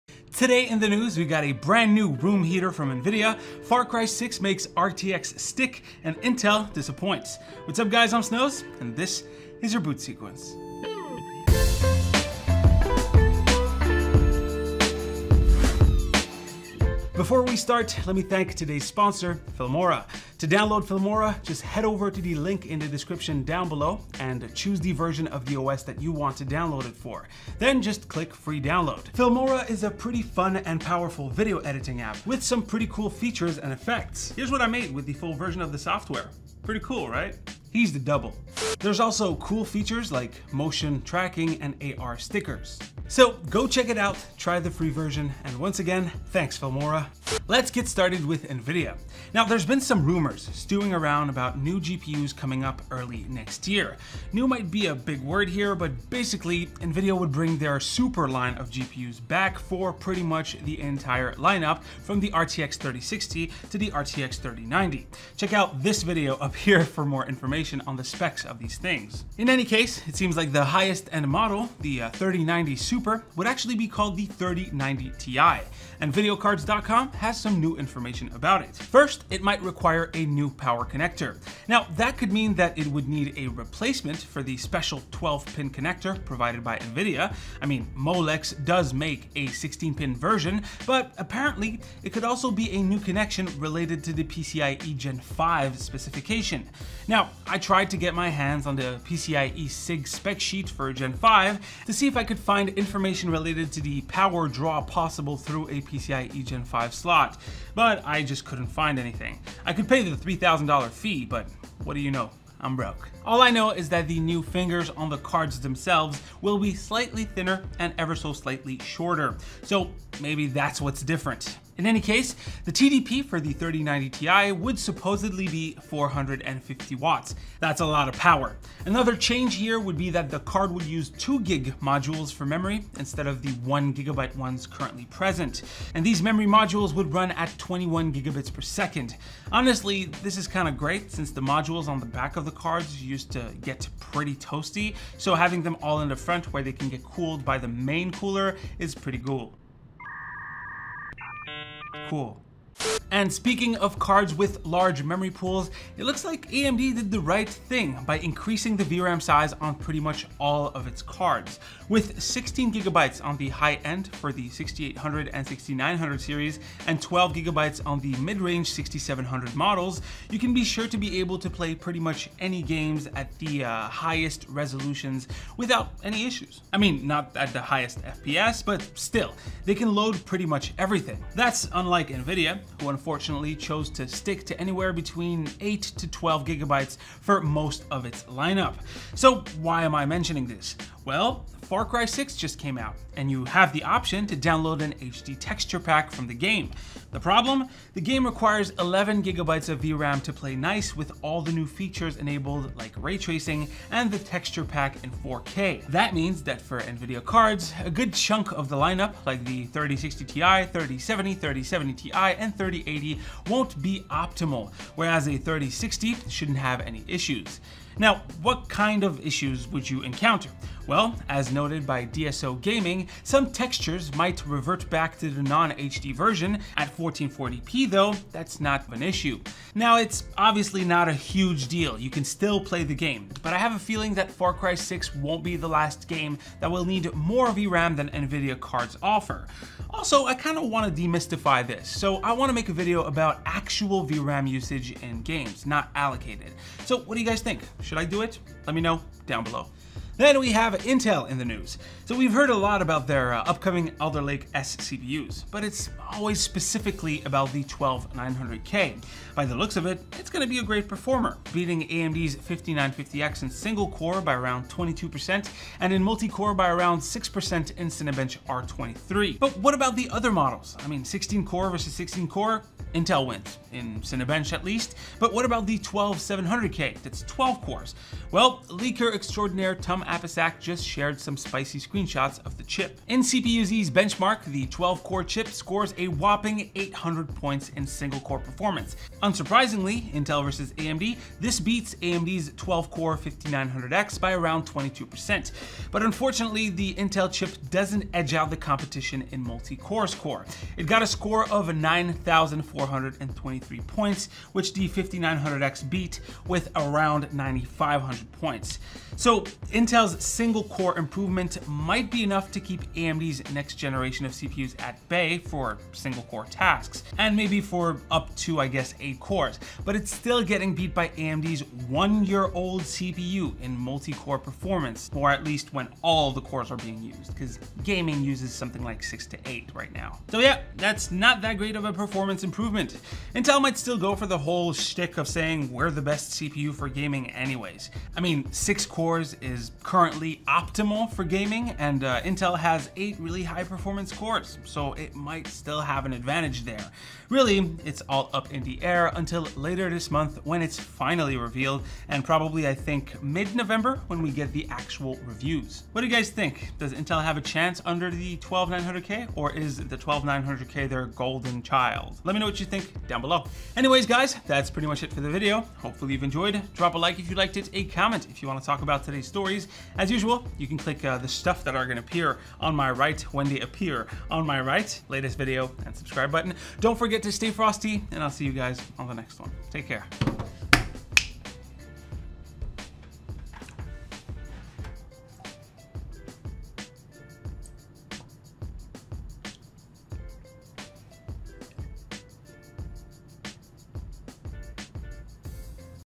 THIS is Why Nvidia Should Add MORE Vram!
Windows 10 Pro Oem + Office 2019 Pro Plus Key Pack ($50)

It looks like AMD had the right idea by increasing the Vram size on pretty much all of its cards. With 16gb on the high end with the 6800 and 6900 series, and 12gb on the midrange 6700 models, you can be sure to be able to play pretty much any games at the highest resolutions without any issues. I mean not at the highest FPS but still, they can load pretty much everything.

That’s Unlike Nvidia who unfortunately chose to stick to anywhere from 8 to 12GB for most of its models. So why am I mentioning this ? Well, Far cry 6 just came out, and you have the option to download an HD Texture pack for the game. The problem ? The game requires 11GB of Vram to play nice with all the new features enabled like ray tracing, and the texture pack in 4k. That means that for Nvidia cards, a good chunk of the lineup like the 3060 ti, 3070,3070 ti and 3080 wont be optimal. Whereas a 3060 shouldn’t have any issues. Now, What kind of issues ? Well, as noted by DSOgaming, some textures might revert back to the non HD versions and some might simply fail to load at 4k. At 1440p thought, that’s not too much of an issue.